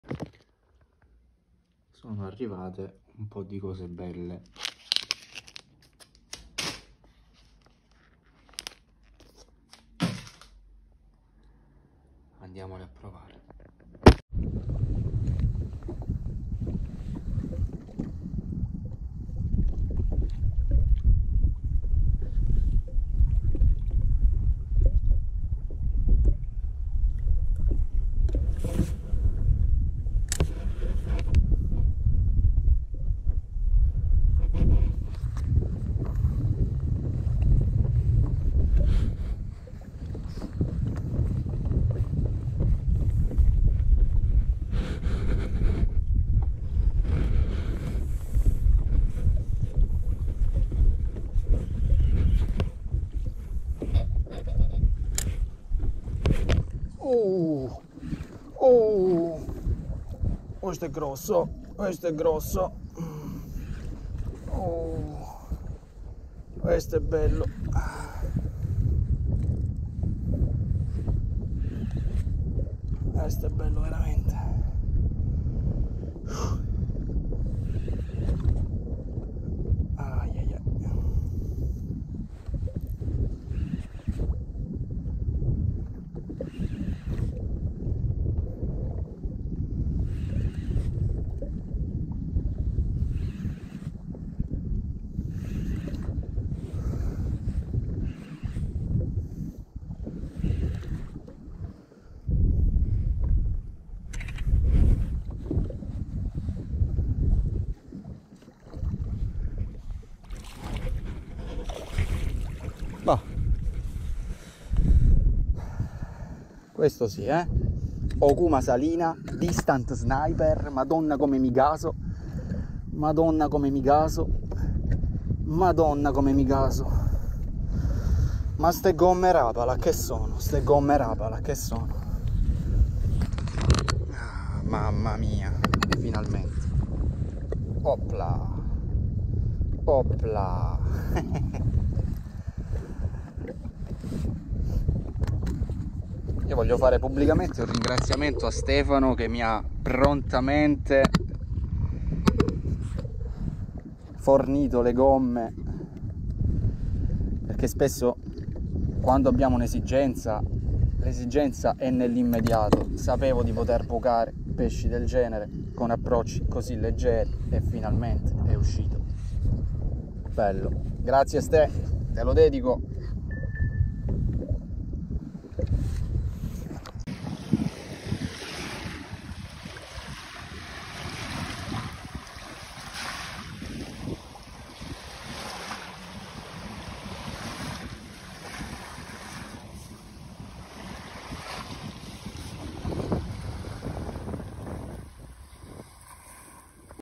SOFTBAITS RAPALA a bordo del WILDCAT FX: DENTICE con STRIKE in diretta
Veloce sessione di pesca al dentice a bordo del Wildcat di @GalaxykayaksEu @bolsenayachtingsport
In verticale, in leggerezza con le softbaits di casa @RapalaVmcItalia, le CRUSHCITY KICKMAN.

Attrezzatura utilizzata:
@Rapala Distant Sniper
@okumafishingitaliaOkuma Salina 4000H
Sufix Lines 832 + FC Invisiline